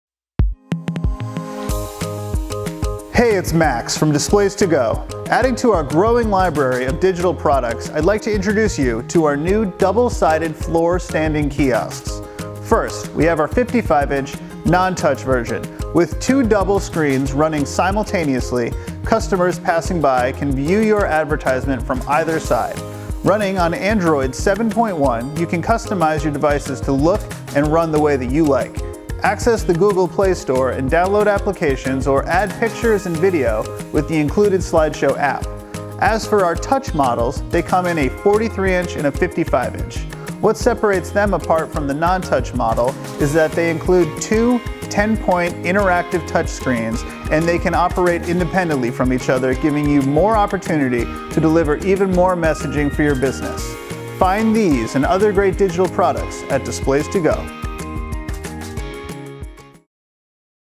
Double-Sided Digital Signage & Touch Screen Kiosks | Displays2go®
Max showcases the design and features of Displays2go's double-sided digital kiosks. Running on the latest version of Android OS for digital signage, these LCD displays are equipped to create personalized digital experiences for any market at an affordable price. No contracts, fees or unnecessary quote requests. Order online and your kiosk ships same day.

Our non-touch 55" models allow businesses to run multimedia advertisements in sync with increased visibility. Shoppers, visitors and students can view promotional offers, building directories, or important information when passing by the sign from either direction. This means less missed opportunities for your content to be seen.

Available with a 43" or 55" screen, our touchscreen kiosks deliver interactive experiences on two independently operating displays. The dual 10pt IR touch screens save floor space while creating a convenient and efficient touchpoint for multiple users. With Android OS, the possibilities for offering smart customer-oriented solutions are endless. Load any mobile-optimized website on the pre-installed browser, download apps from the Google Play Store, or even upload your own custom-built applications directly to the device.